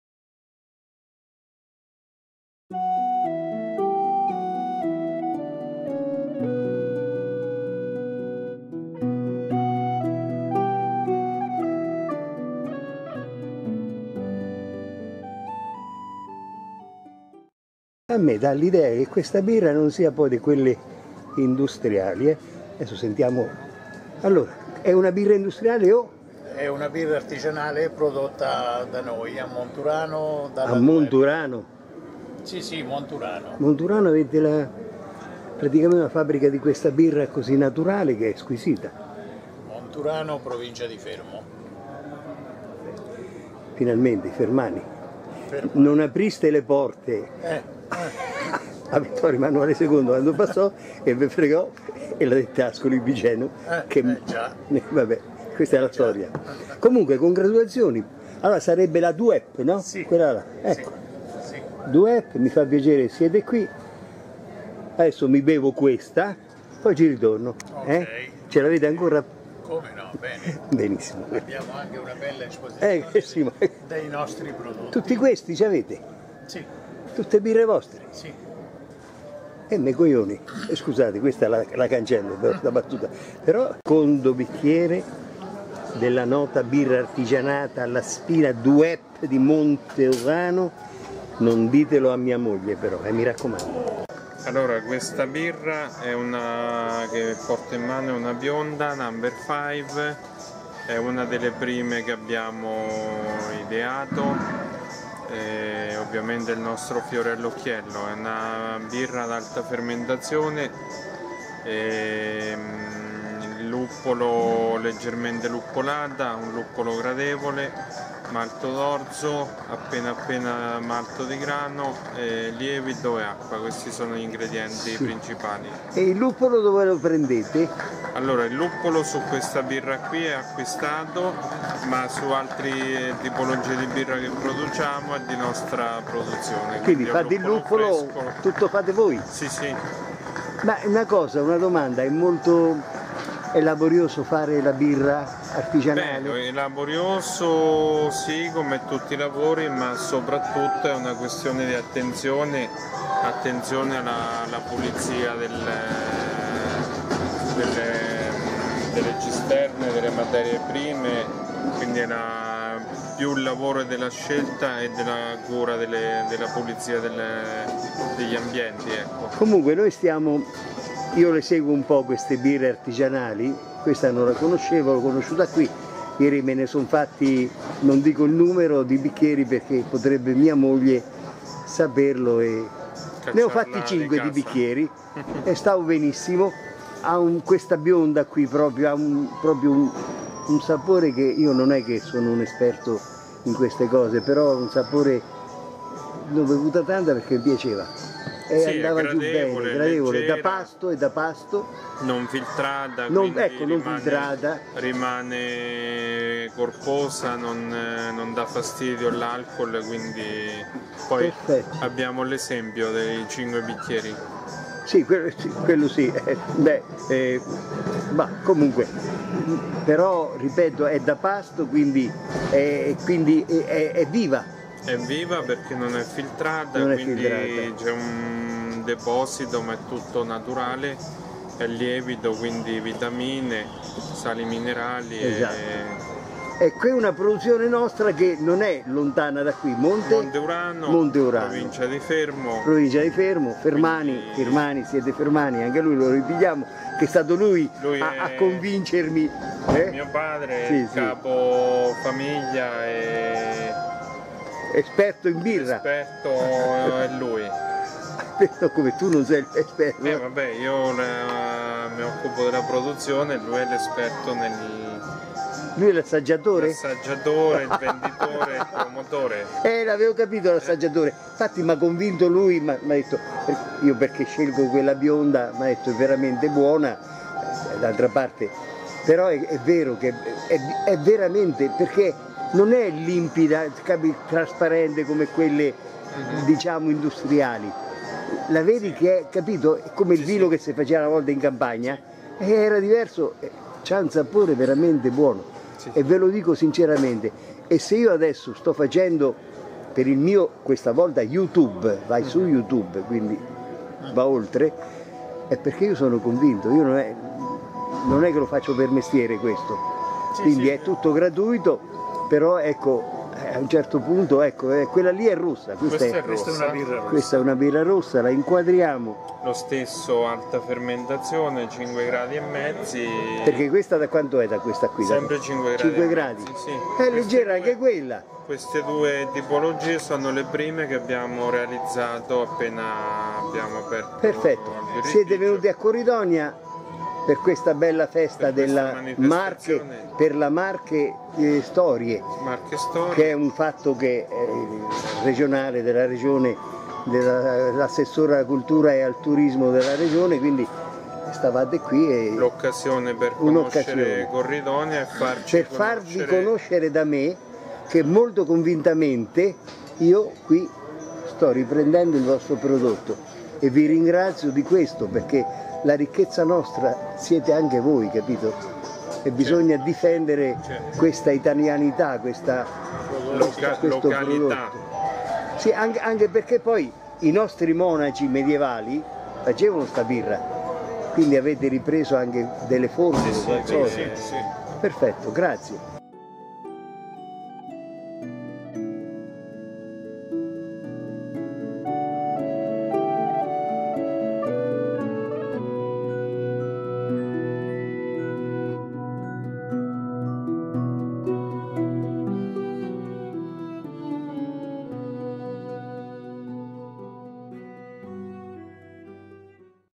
MARCHE STORIE: La Birra Artigianale "DUE P" al Borgo di Montolmo il 9 - 10 - 11 settembre 2022
Questa birra artiginale marchigiana mi è piaciuta e quindi le ho dato un riconoscimento pubblico